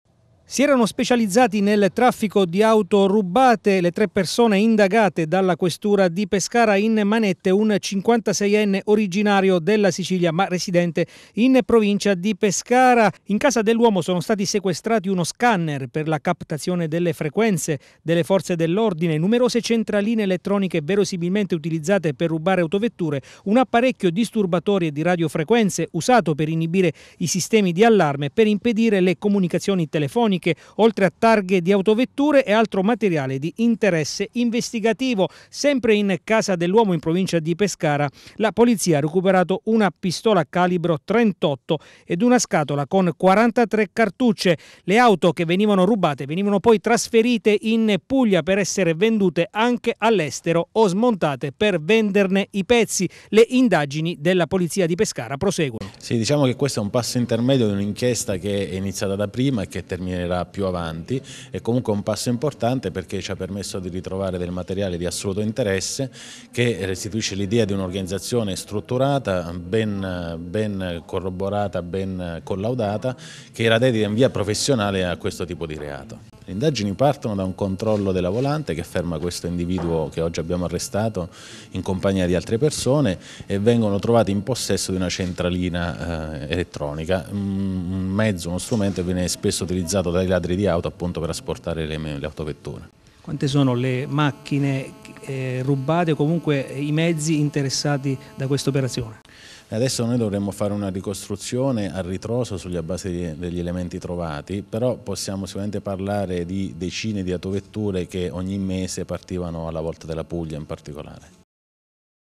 Sgominata organizzazione specializzata nei furti di auto
La Squadra Mobile della Questura di Pescara ha sgominato una organizzazione criminale specializzta nei furti auto